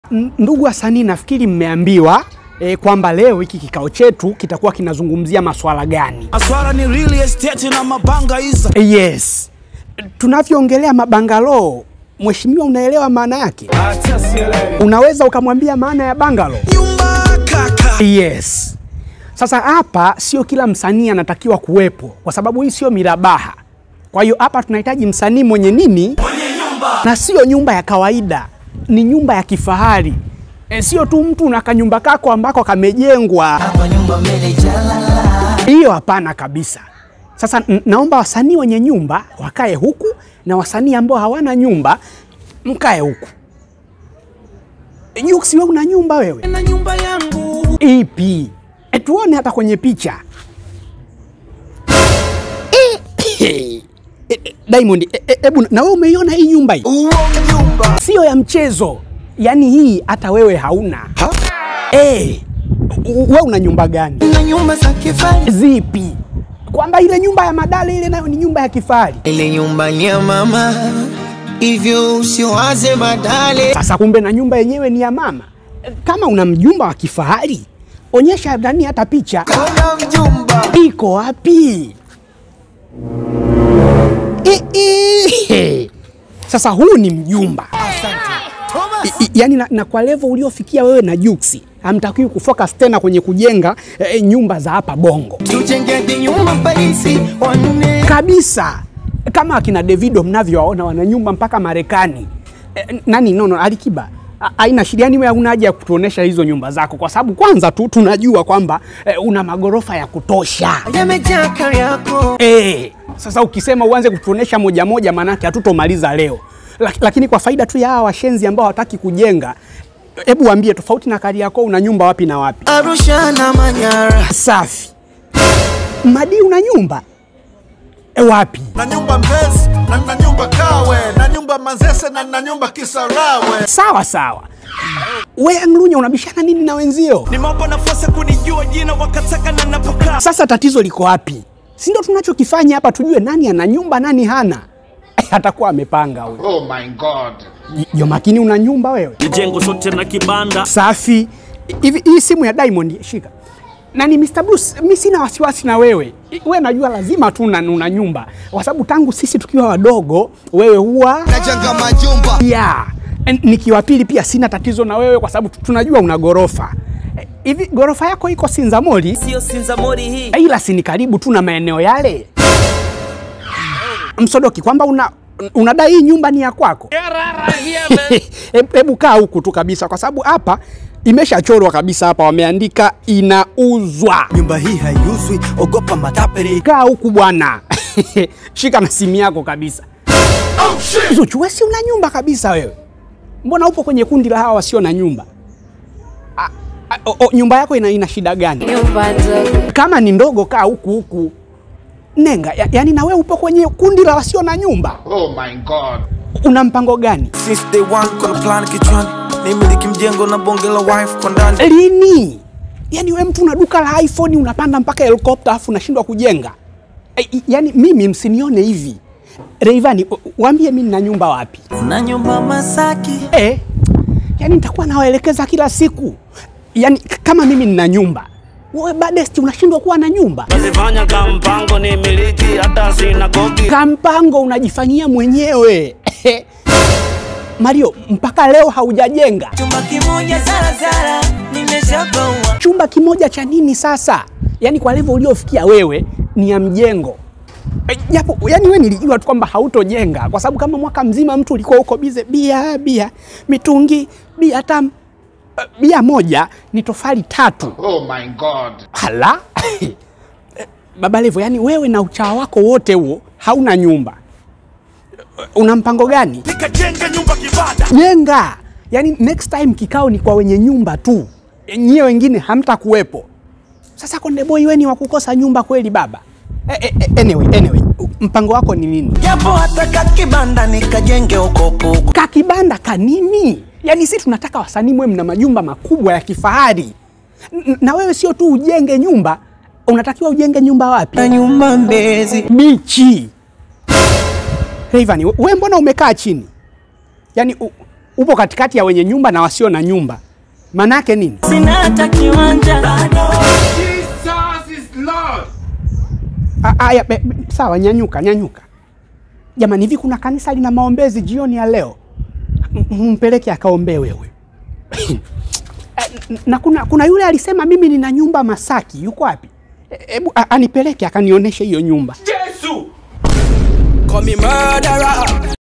WASANII WENYE NYUMBA KALI - USWEGE MURDERER
Uswege afanya kikao na Wasanii wa Bongo wenye Majumba makali, Rayvanny hana hata kiwanja.